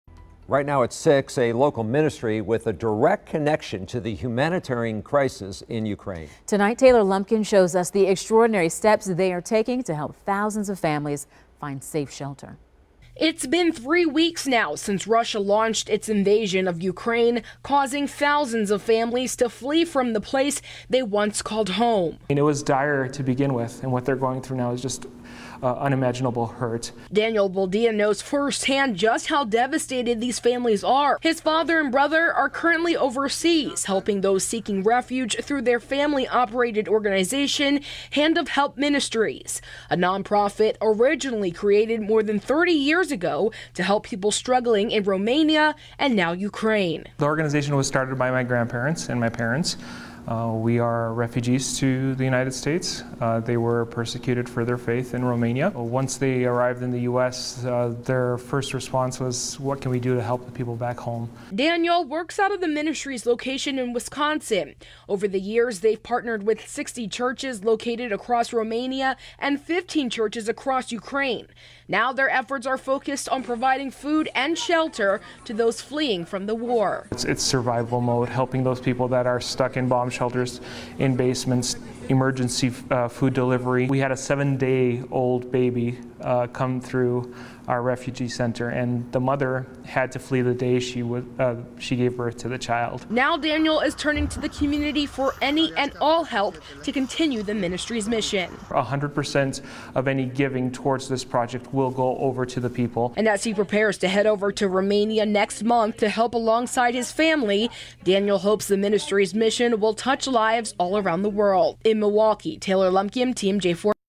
Local ministry steps up to raise funds to help families fleeing from Ukraine
It's been three weeks now since Russia launched its invasion of Ukraine causing thousands of families to flee from the place they once called home.